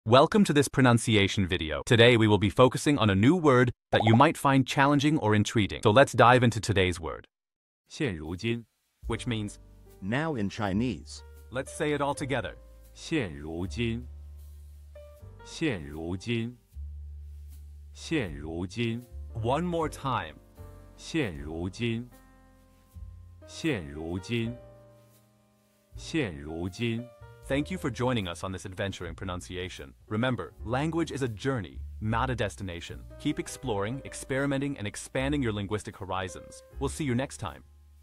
How to pronounce xiàn rú jīn | 现如今 (Now in Chinese)
Master the Pronunciation of 'xiàn rú jīn | 现如今' - which means : Now in Chinese 📝 with @PerfectPronunciationChinese 🎓🗣️ - [Your Guide to Chinese]

Learn to pronounce 'xiàn rú jīn' perfectly in Chinese with our easy and engaging tutorial by @PerfectPronunciationChinese! 📖📽️ In this comprehensive video, we provide you with native speaker examples 🗣️👂 and phonetic breakdowns 🎵 to ensure you get the pronunciation just right.